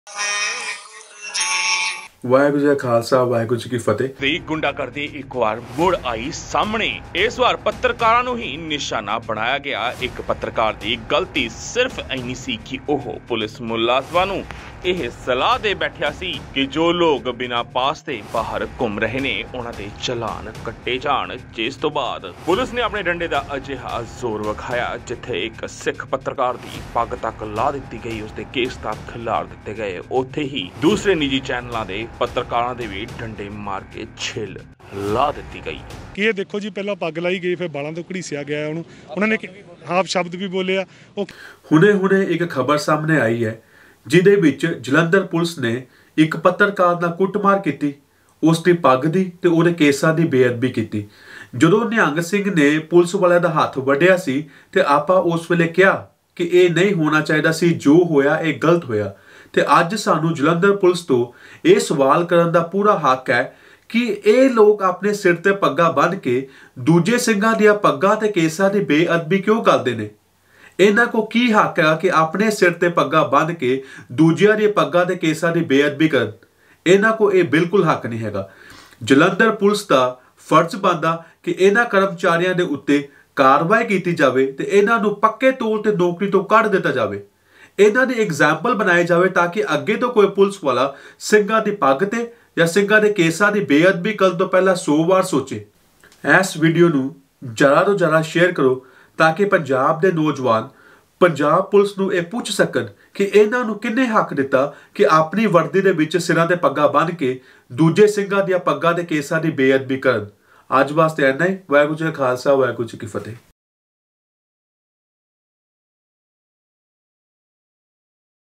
Punjab Police disrespect turban again || should punjab police wear turban as part of their uniform?
In this video, jalandhar police beat up a reporter, took his turban off and dragged him from his hair. Should punjab police be allowed to wear turban ? Waheguru Ji Ka khalsa Waheguru ji Ki Fateh.